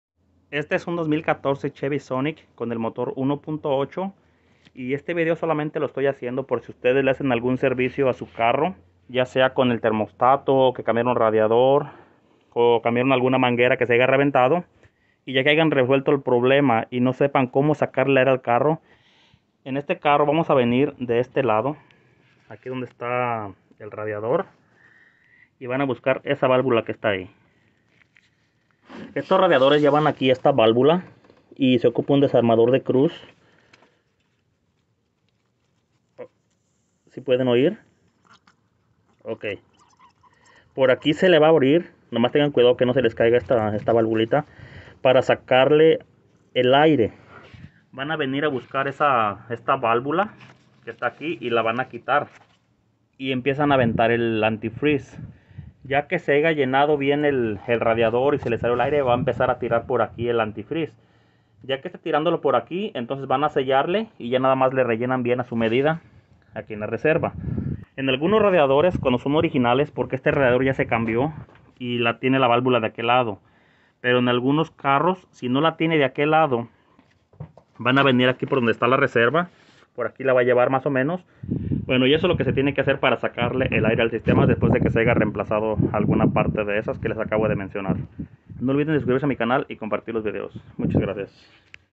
2014 chevy sonic 1.8 Cómo purgar el sistema de enfriamiento (how to bleed cooling system)
Este es un 2014 Chevy Sonic y este video solamente lo hago para así hacen un servicio en su carro y no saben Cómo sacarle el aire al sistema de enfriamiento Espero que este video les ayude a guiarlos cómo pueden hacerlo. no olviden de suscribirse a mi canal y compartir los videos Muchas gracias.

This is a 2014 Chevy Sonic and I'm making this video to show you how to get the air out of your car's cooling system.